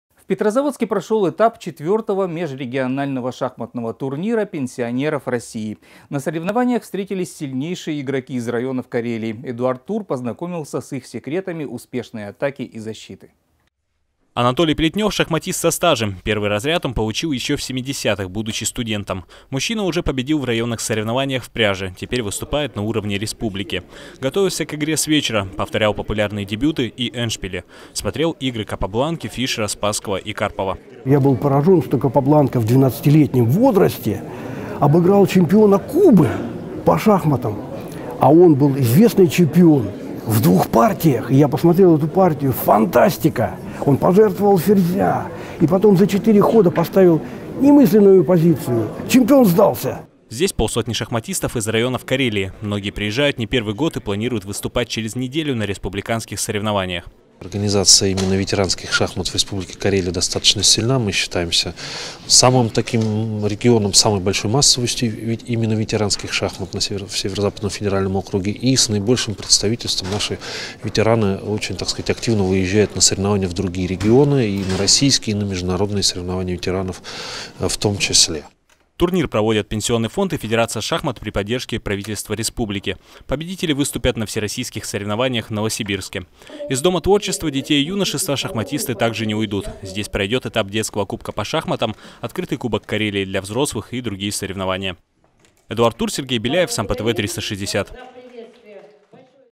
Шахматам все возраста покорны.
В Петрозаводске прошел этап “Межрегионального шахматного турнира пенсионеров России”. Встретились сильнейшие игроки из районов.